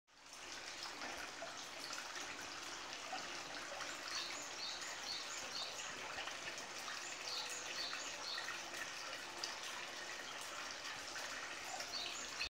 高湯温泉 花月ハイランドホテル「貸切風呂」
貸切りでこの広さと解放感。格別な風呂です。